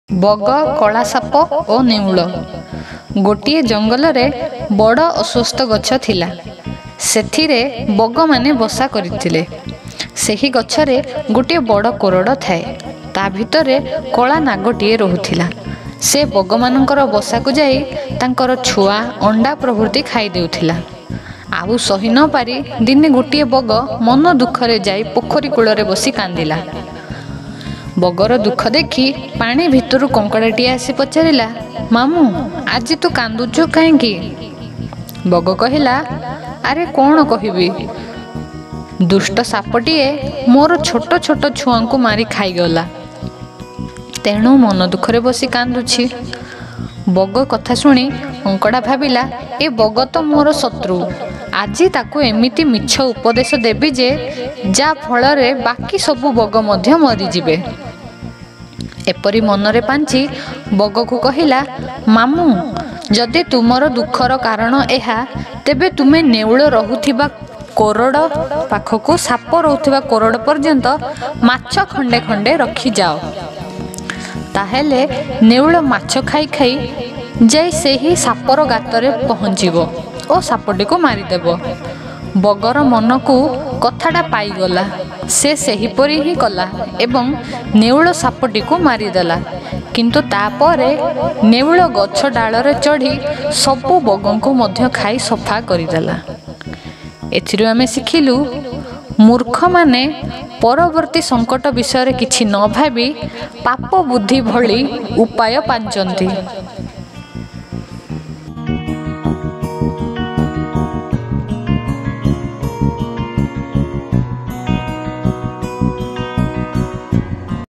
ବଗ, କଳାସାପ ଓ ନେଉଳ! BAGA, KALASAPA O NEULA! Odia moral story ! Panchatantra
Panchatantra stories are one of the most widely translated books in the history and are known for their wisdom on practical life. The stories themselves are delightfully narrated, with animals and birds often being the central characters. Thus they provide valuable life lessons in a light-hearted manner.
This tale teaches us once someone's trust is broken how difficult it is to get it back. Lying can help you survive only for a short time, but if it becomes a regular habit you can easily lose friends in the long run.